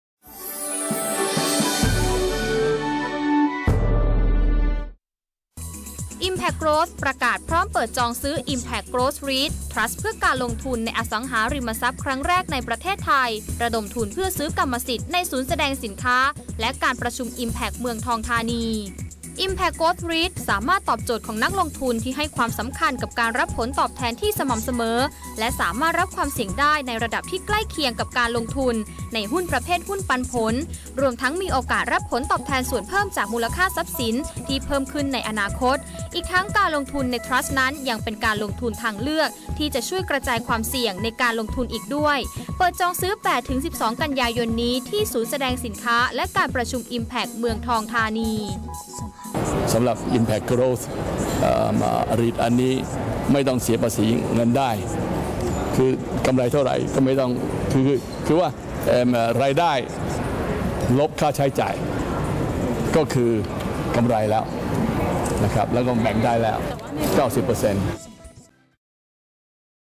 IMPACT Growth REIT, by Bright News 08 09 57
บริษัท อาร์ เอ็ม ไอ จำกัด ได้ยื่นคำขอจัดตั้งทรัสต์เพื่อการลงทุนในอสังหาริมทรัพย์อิมแพ็คโกรท (IMPACT GROWTH REIT หรือ IMPACT) โดย IMPACT เป็นทรัสต์เพื่อการลงทุนในอสังหาริมทรัพย์ที่จะเข้าลงทุนในกรรมสิทธิ์ ทรัพย์สินประเภทศูนย์แสดงสินค้าและศูนย์การประชุม ที่ตั้งอยู่ในโครงการอิมแพ็ค เมืองทองธานี อันได้แก่ IMPACT ARENA, IMPACT CHALLENGER, IMPACT FORUM และ IMPACT EXHIBITION CENTER โดยจะมีการออกและเสนอขายหน่วยทรัสต์ของ IMPACT ภายหลังจากได้รับอนุญาตให้ออกและเสนอขายหน่วยทรัสต์จากสำนักงานคณะกรรมการกำกับหลักทรัพย์และตลาดหลักทรัพย์

โดยมีบริษัท อาร์ เอ็ม ไอ จำกัด จะทำหน้าที่เป็นผู้จัดการกองทรัสต์ (REIT Manager)บริษัทหลักทรัพย์จัดการกองทุน กสิกรไทย จำกัด ทำหน้าที่เป็นทรัสตี(Trustee) บริษัทหลักทรัพย์ เมย์แบงก์ กิมเอ็ง (ประเทศไทย) จำกัด (มหาชน) เป็นที่ปรึกษาทางการเงิน และบริษัท อิมแพ็ค เอ็กซิบิชั่น แมเนจเม้นท์ จำกัด ทำหน้าที่เป็นผู้บริหารอสังหาริมทรัพย์ (Property Manager) ในด้านการเสนอขายหน่วยทรัสต์บริษัท อาร์ เอ็ม ไอ จำกัด ได้แต่งตั้ง ธนาคารกสิกรไทย จำกัด (มหาชน) บริษัทหลักทรัพย์ กสิกรไทย จำกัด (มหาชน) และบริษัทหลักทรัพย์ เมย์แบงก์ กิมเอ็ง (ประเทศไทย) จำกัด (มหาชน) เป็นผู้จัดการการจัดจำหน่ายหลักทรัพย์ร่วม (Joint Lead-Underwriters) ในการเสนอขายหน่วยทรัสต์ของ IMPACT ในครั้งนี้เงินที่ได้จากการออกและเสนอขายหน่วยทรัสต์รวมถึงเงินกู้ยืมจากสถาบันการเงิน (หลังจากหักค่าใช้จ่ายและค่าธรรมเนียมต่างๆที่เกี่ยวข้องแล้ว) จะนำไปลงทุนในกรรมสิทธิ์ทรัพย์สินประเภทศูนย์การแสดงและการประชุมที่ตั้งอยู่ในโครงการอิมแพ็คเมืองทองธานี คือ IMPACT ARENA, IMPACT CHALLENGER, IMPACT FORUM และ IMPACT EXHIBITION CENTER